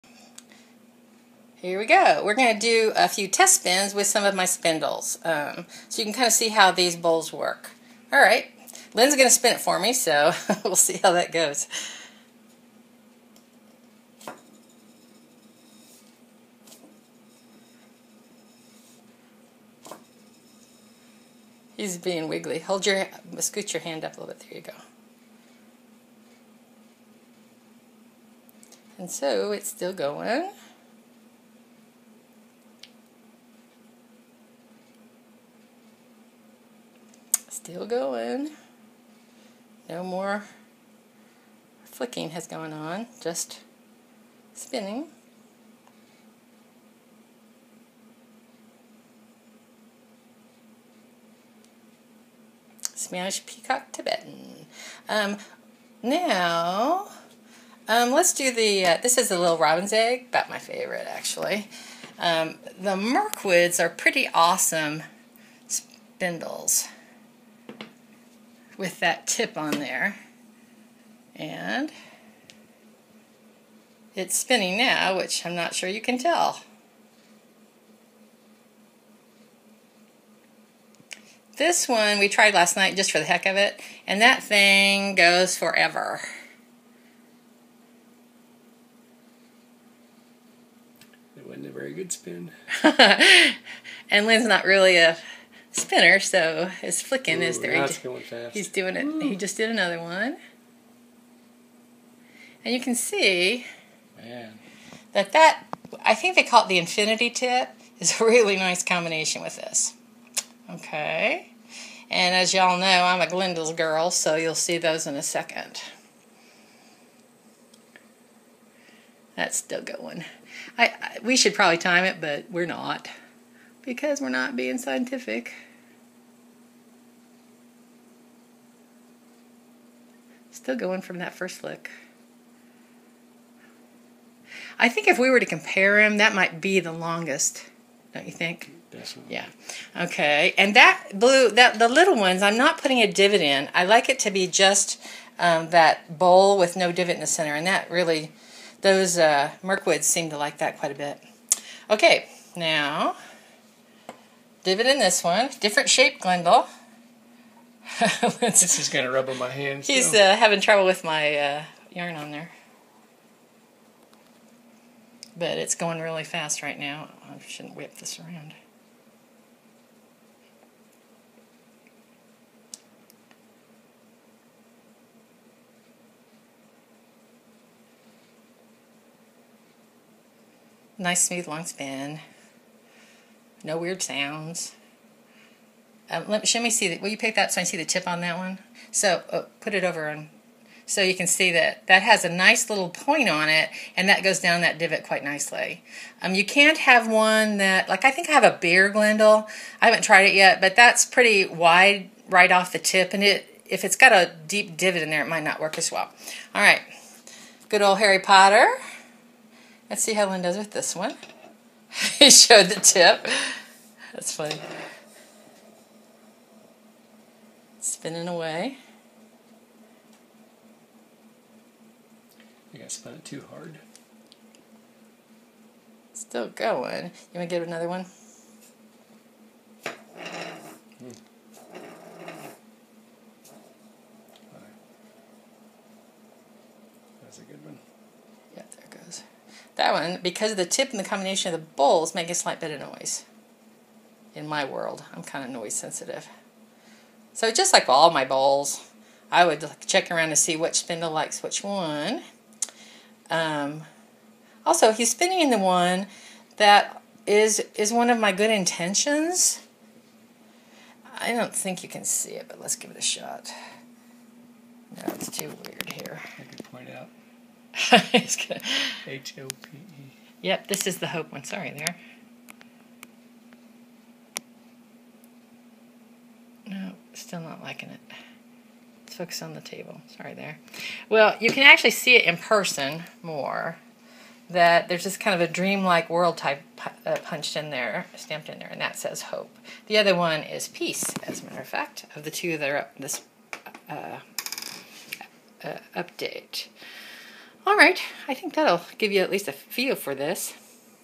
Deb & Lynn Erikson on DebsFibers on Etsy. Test spinning in the new enamel spindle towers.
These are some brief test spins in our new spindle towers with enamel insets. There is a variety of dimple shape in the bottom and one that is a smaller, deeper bowl with no dimple. It's just a process of matching your spindle to the bowl.